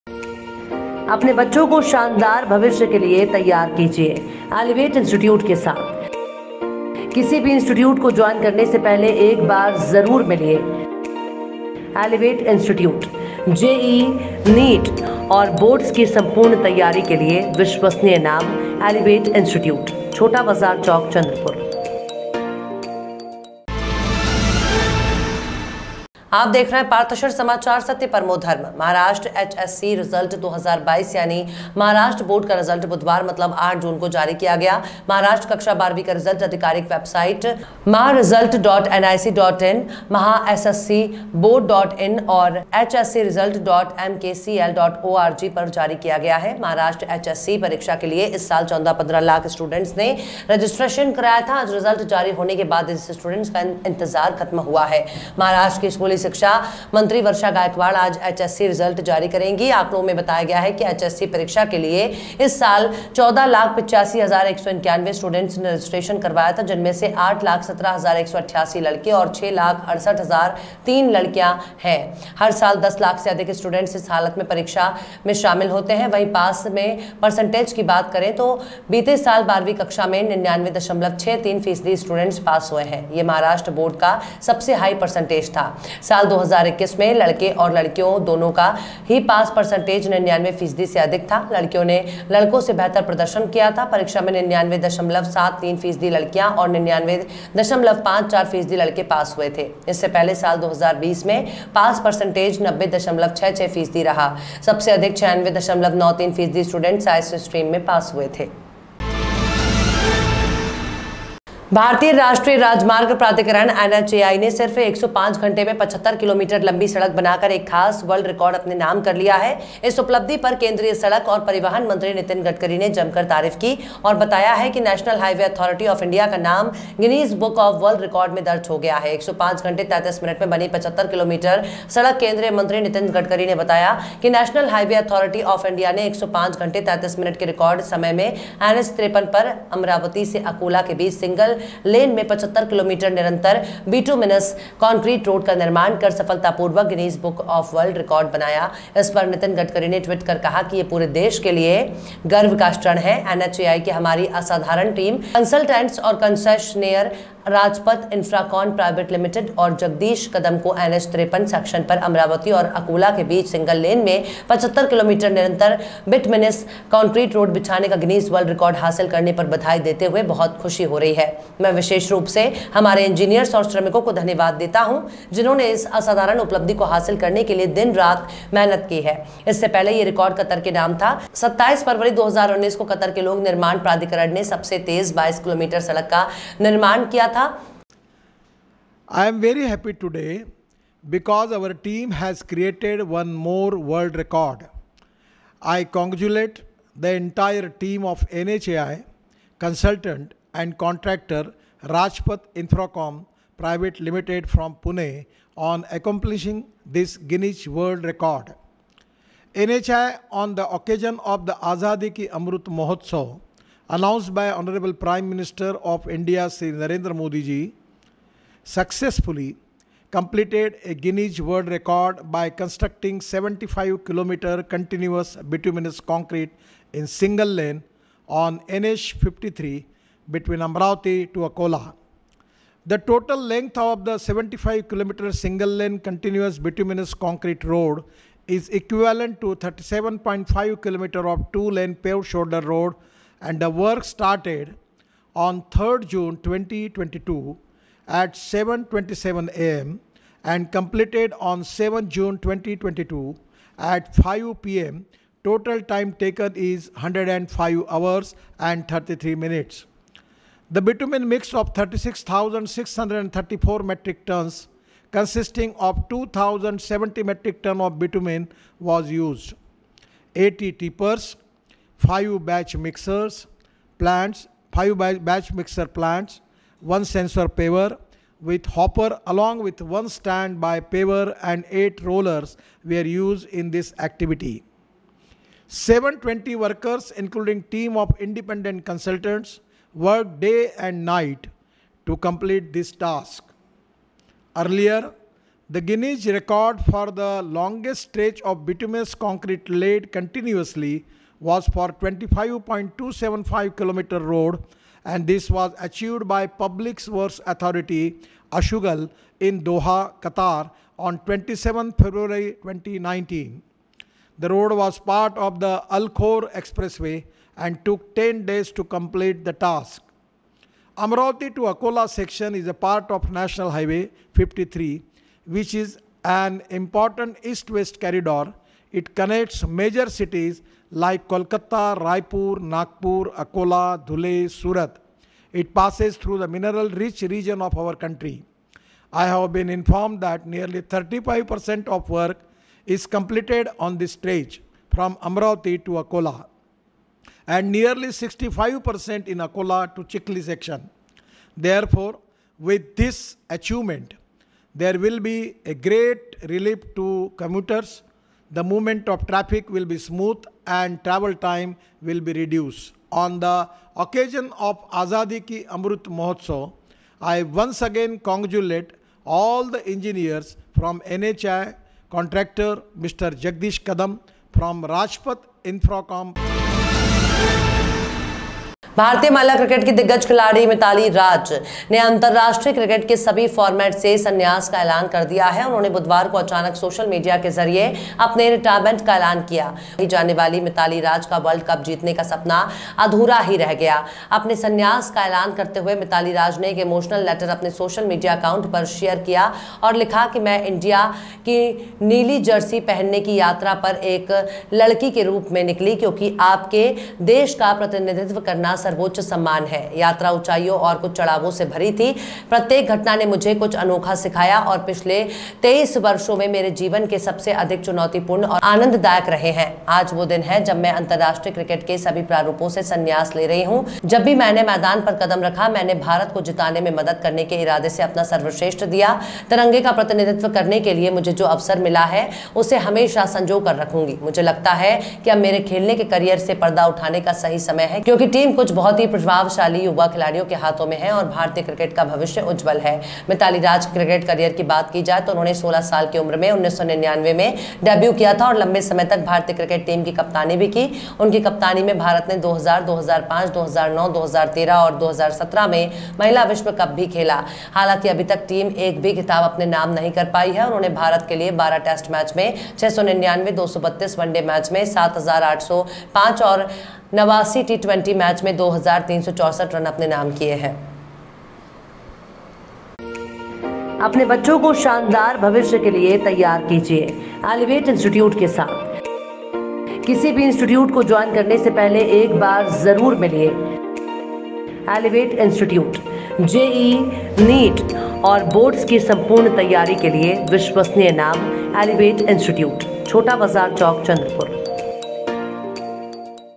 इस खिलाड़ी ने किया क्रिकेट को अलविदा।महाराष्ट्र 12 बोर्ड हुआ प्रकाशित।महाराष्ट्र में बना दमदार रोड।
*महाराष्ट्र 12 बोर्ड हुआ 1 बजे प्रकाशित, इस लिंक के माध्यम से होगा रिजल्ट प्रकाशित*

महाराष्ट्र एचएससी रिजल्ट 2022 यानी महाराष्ट्र बोर्ड का रिजल्ट बुधवार मतलब 8 जून को जारी किया गया. महाराष्ट्र कक्षा 12वीं का रिजल्ट आधिकारिक वेबसाइट mahresult.nic.in , mahahsscboard.in और hscresult.mkcl.org पर जारी किया गया है. महाराष्ट्र एचएससी परीक्षा के लिए इस साल 14-15 लाख स्टूडेंट्स ने रजिस्ट्रेशन कराया था. आज रिजल्ट जारी होने के बाद इन स्टूडेंट्स का इंतजार खत्म हुआ है। महाराष्ट्र की स्कूली शिक्षा मंत्री वर्षा गायकवाड़ आज एचएससी रिजल्ट जारी करेंगी. आंकड़ों में बताया गया है कि एचएससी परीक्षा के लिए इस साल 14,85,191 स्टूडेंट्स ने रजिस्ट्रेशन करवाया था, जिनमें से 8,17,188 लड़के और 6,68,003 लड़कियां हैं. हर साल 10 लाख से अधिक स्टूडेंट्स इस परीक्षा में शामिल होते हैं. वहीं पास परसेंटेज की बात करें, तो बीते साल 12वीं कक्षा में 99.63 फीसदी स्टूडेंट पास हुए थे. यह महाराष्ट्र बोर्ड का सबसे हाई परसेंटेज था. साल 2021 में लड़के और लड़कियों दोनों का ही पास परसेंटेज 99 फीसदी से अधिक था. लड़कियों ने लड़कों से बेहतर प्रदर्शन किया था. परीक्षा में 99.73 फीसदी लड़कियां और 99.54 फीसदी लड़के पास हुए थे. इससे पहले साल 2020 में पास परसेंटेज 90.66 फीसदी रहा था. सबसे अधिक 96.93 फीसदी स्टूडेंट साइंस स्ट्रीम में पास हुए थे.

 *महाराष्ट्र में बना यह दमदार रोड, जिसने बनाया वर्ल्ड रिकॉर्ड*

भारतीय राष्ट्रीय राजमार्ग प्राधिकरण NHAI ने सिर्फ 105 घंटे में 75 किलोमीटर लंबी सड़क बनाकर एक खास वर्ल्ड रिकॉर्ड अपने नाम कर लिया है. इस उपलब्धि पर केंद्रीय सड़क एवं परिवहन मंत्री नितिन गडकरी ने जमकर तारीफ की और बताया है कि नेशनल हाईवे अथॉरिटी ऑफ इंडिया का नाम गिनीज बुक ऑफ वर्ल्ड रिकॉर्ड में दर्ज हो गया है. 105 घंटे 33 मिनट में बनी 75 किलोमीटर सड़क केंद्रीय मंत्री नितिन गडकरी ने बताया कि नेशनल हाईवे अथॉरिटी ऑफ इंडिया  ने 105 घंटे 33 मिनट के रिकॉर्ड समय में NH-53 पर अमरावती से अकोला के बीच सिंगल लेन में 75 किलोमीटर निरंतर बिटुमिनस कंक्रीट रोड का निर्माण कर सफलतापूर्वक गिनीज वर्ल्ड रिकॉर्ड बनाया.

इसपर नितिन गडकरी ने ट्वीट कर कहा, 'यह पूरे देश के लिए गर्व का क्षण है. NHAI की हमारी असाधारण टीम, कंसल्टेंट्स और कंसेशनेयर, राजपथ इंफ्राकॉन प्राइवेट लिमिटेड और जगदीश कदम को NH-53 सेक्शन पर अमरावती और अकोला के बीच सिंगल लेन में 75 किलोमीटर निरंतर बिटुमिनस कंक्रीट रोड बिछाने का गिनीज वर्ल्ड रिकॉर्ड हासिल करने पर बधाई देते हुए बहुत खुशी हो रही है. मैं विशेष रूप से हमारे इंजीनियर्स और श्रमिकों को धन्यवाद देता हूं, जिन्होंने इस असाधारण उपलब्धि को हासिल करने के लिए दिन-रात मेहनत की.

 इससे पहले यह रिकॉर्ड कतर के नाम था. 27 फरवरी 2019 को कतर के लोक निर्माण प्राधिकरण ने सबसे तेज 22 किलोमीटर सड़क का निर्माण किया था.

*महिला सचिन तेंदुलकर ने किया क्रिकेट फील्ड को अलविदा, जानिए कारण*

भारतीय महिला क्रिकेट की दिग्गज खिलाड़ी मिताली राज (Mithali Raj) ने अंतरराष्ट्रीय क्रिकेट के सभी फॉर्मेट से संन्यास का ऐलान कर दिया है। उन्होंने बुधवार को अचानक सोशल मीडिया के जरिए अपने रिटायरमेंट का ऐलान किया।

39 वर्षीय इस क्रिकेटर ने भारतीय टीम के लिए टेस्ट, वनडे, टी-20 तीनों फॉर्मेट में कप्तानी की है। लेकिन लेडी सचिन तेंदुलकर कही जाने वाली मिताली राज का वर्ल्ड कप जीतने का सपना अधूरा ही रह गया।
अपने संन्यास का ऐलान करते हुए मिताली राज ने एक इमोशनल लेटर अपने सोशल मीडिया अकाउंट पर शेयर किया और लिखा- "मैं इंडिया की नीली जर्सी पहनने की यात्रा पर एक छोटी लड़की के रूप में निकली, क्योंकि आपके देश का प्रतिनिधित्व करना सर्वोच्च सम्मान है। यात्रा ऊंचाइयों और कुछ चढ़ावों से भरी थी। प्रत्येक घटना ने मुझे कुछ अनोखा सिखाया और पिछले 23 वर्ष मेरे जीवन के सबसे अधिक चुनौतीपूर्ण और आनंददायक रहे हैं। आज वह दिन है जब मैं अंतरराष्ट्रीय क्रिकेट के सभी प्रारूपों से संन्यास ले रही हूं। जब भी मैंने मैदान पर कदम रखा, मैंने भारत को जीतने में मदद करने के इरादे से अपना सर्वश्रेष्ठ दिया। तिरंगे का प्रतिनिधित्व करने के लिए मुझे जो अवसर मिला है, मैं उसे हमेशा संजो कर रखूंगी। मुझे लगता है कि अब मेरे खेलने के करियर से पर्दा उठाने का सही समय है, क्योंकि टीम कुछ बहुत ही प्रतिभाशाली युवा खिलाड़ियों के हाथों में है और भारतीय क्रिकेट का भविष्य उज्ज्वल है ।

 मिताली राज के क्रिकेट करियर की बात की जाए तो उन्होंने 16 साल की उम्र में 1999 में डेब्यू किया था और लंबे समय तक भारतीय क्रिकेट टीम की कप्तानी भी की। उनकी कप्तानी में भारत ने 2000, 2005, 2009, 2013 और 2017 में महिला विश्व कप भी खेला। हालांकि, अभी तक टीम एक भी खिताब अपने नाम नहीं कर पाई है। उन्होंने भारत के लिए 12 टेस्ट मैच में 699, 232 वनडे मैच में 7805 और 89 टी-20 मैच में 2364 रन अपने नाम की है।